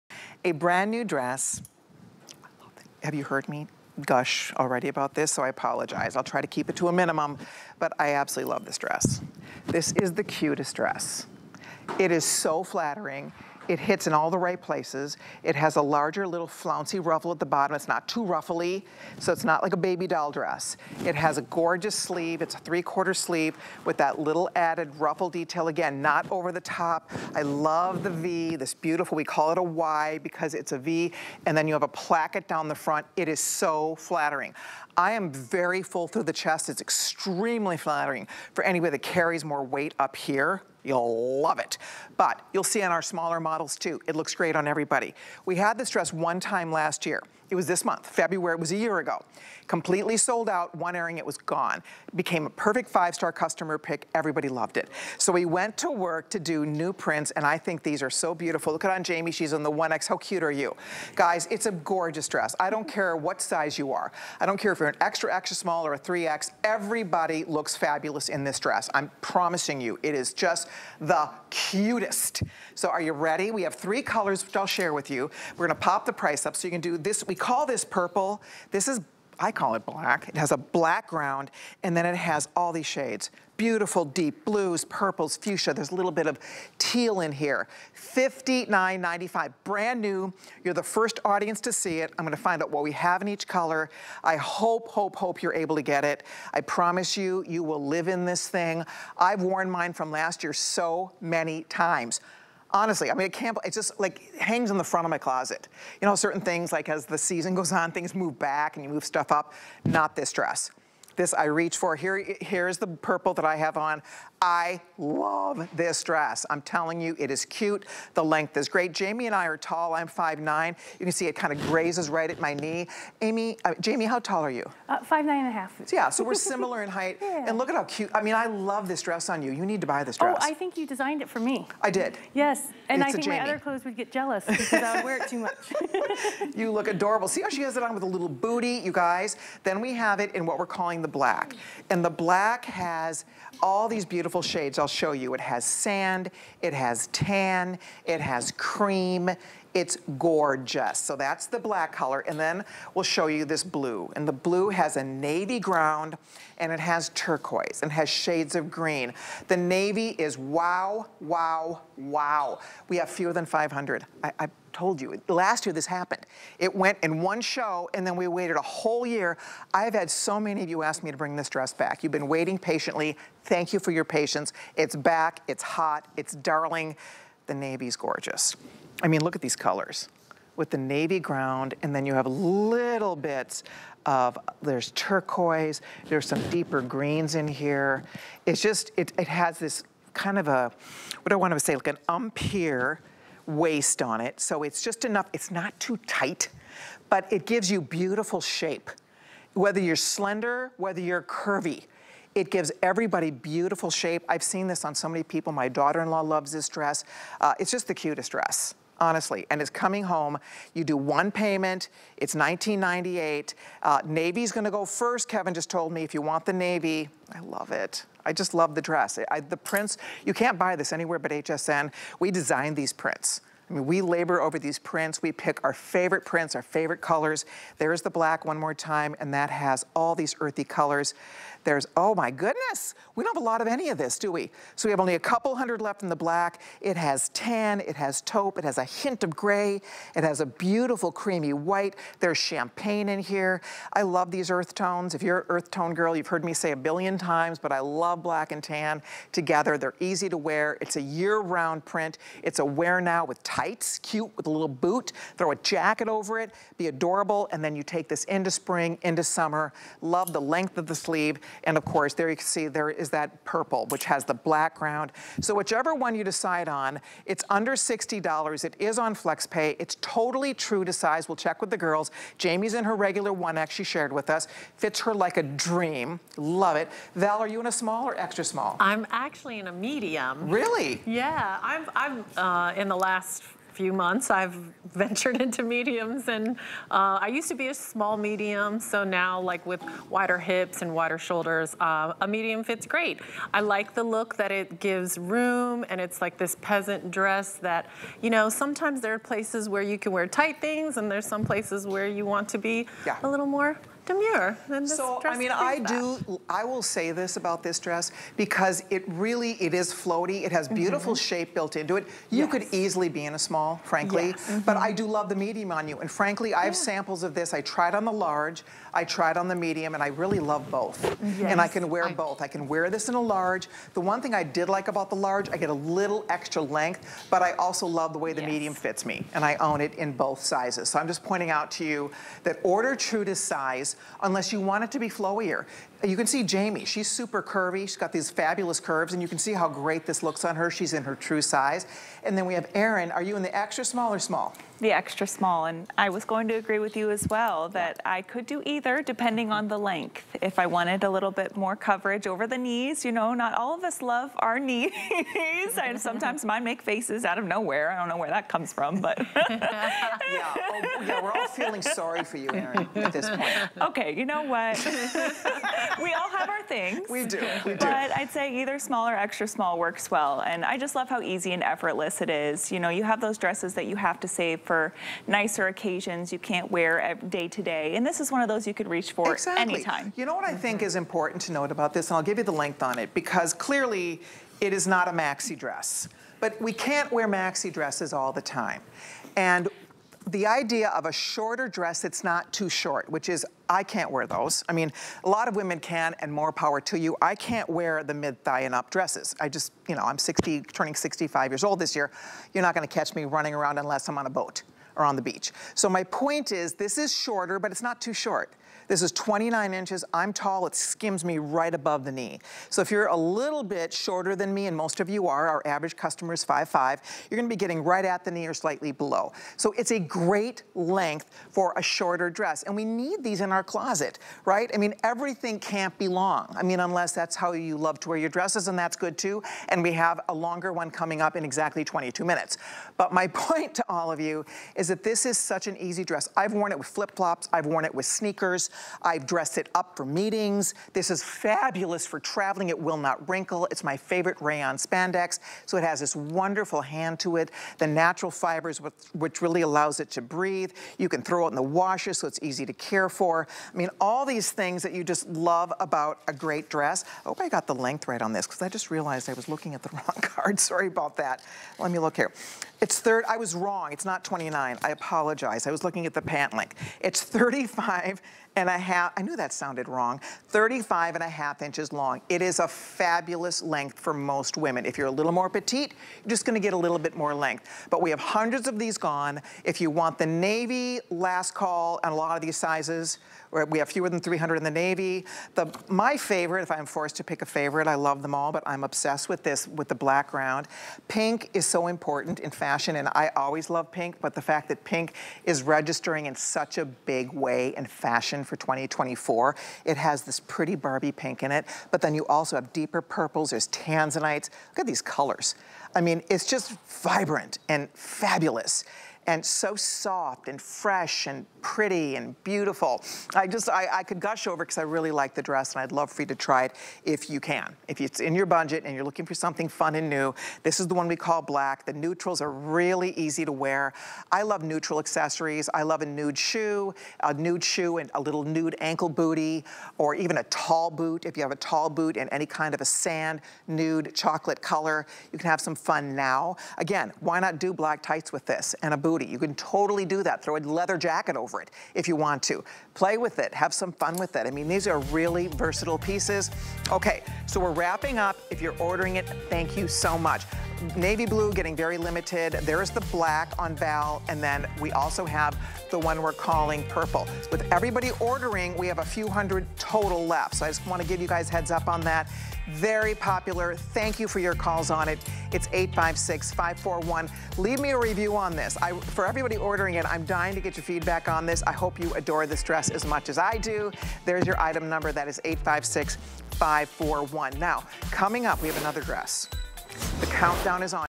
Colleen Lopez Printed Yneck Dress with Tiered Skirt
This chic tiered dress is an easy popover style. It transitions from season to season with sophistication you'll love.